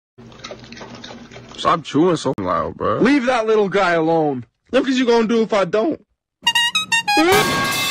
Random characters show: chewing loudly pizza 🍕
Blue really eats pizza but o was mean and Bomby holds a-15 and throws into him